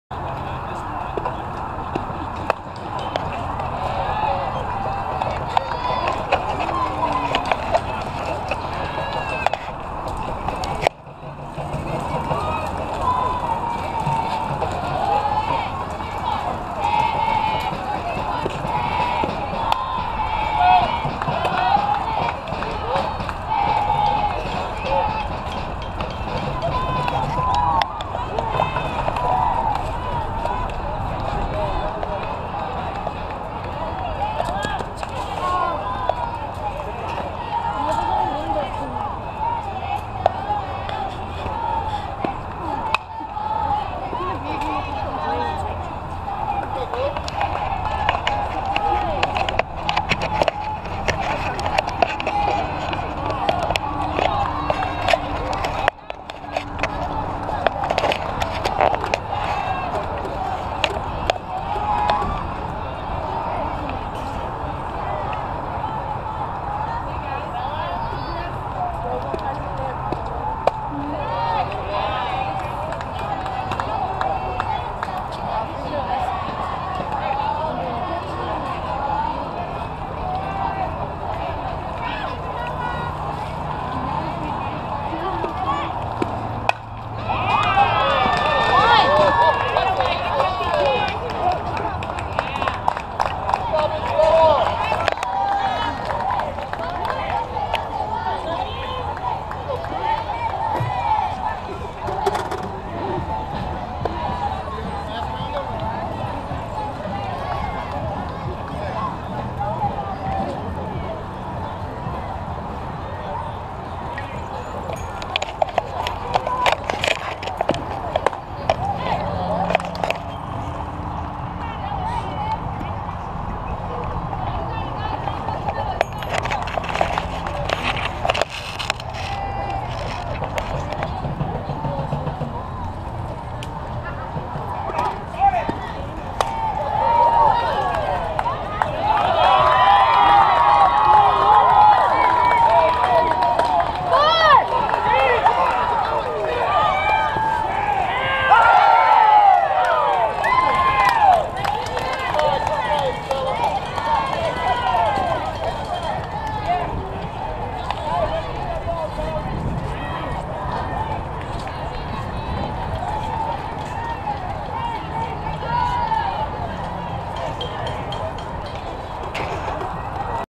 Cooperstown Lady Cobras[TXN] vs Texas Elite LeClair [TXN] GOPR6328
Texas USSSA 2016 San Jancito Day Celebration April 23-24 @ The Ballfields at Craig Ranch McKinney, TX

Pool Play Schedule:12 Open
Game 1
L 4-1